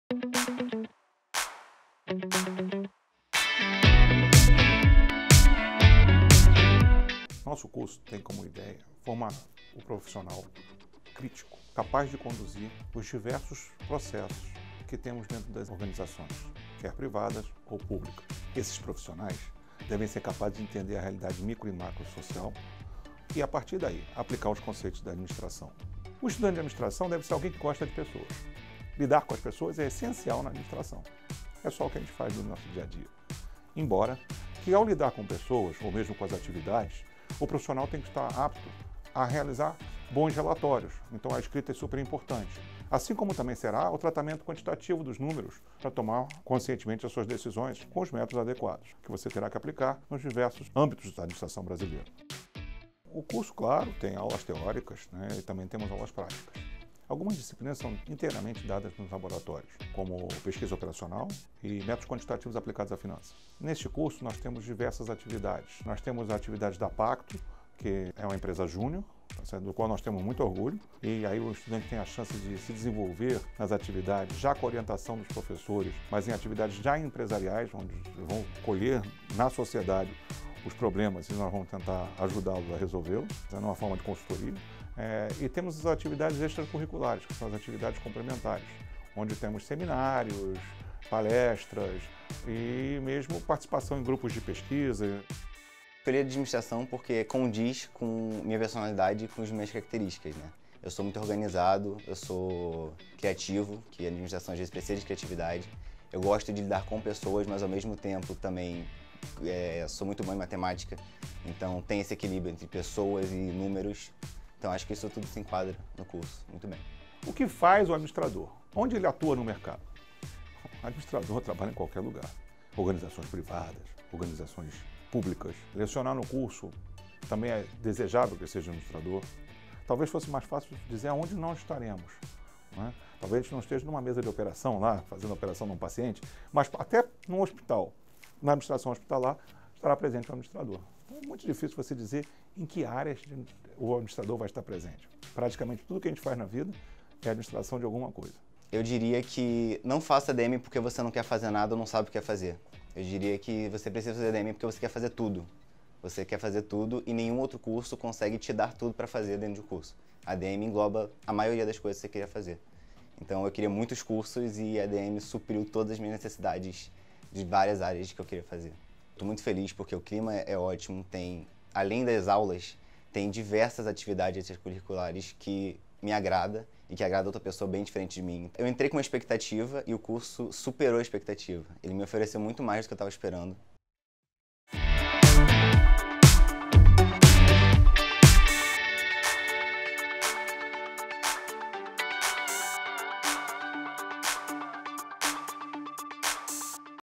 Cursos da UFF: Administração
Muitos dizem que fazer administração é para quem não sabe o que quer, mas na verdade é a versatilidade do curso que o torna atrativo. Entenda mais sobre as possibilidades do administrador com os relatos do professor Ariel Levy e do aluno Igor Dresjan. Este curso está presente nas unidades de Macaé, Niterói e Volta Redonda. O vídeo foi realizado do curso em Niterói.